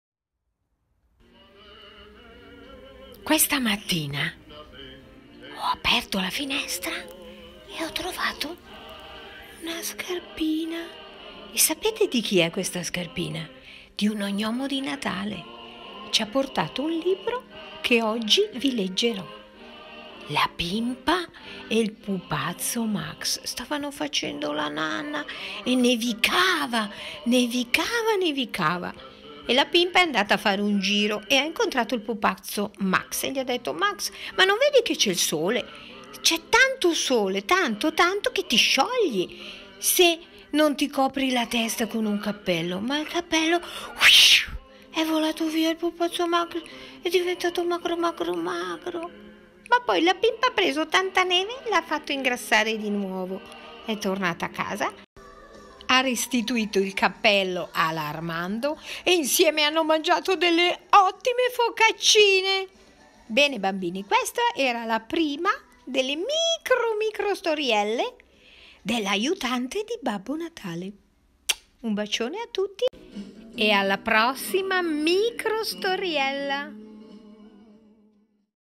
LA PIMPA E IL PUPAZZO MAX
Favole per bambini - Voce narrante Nadia Mentasti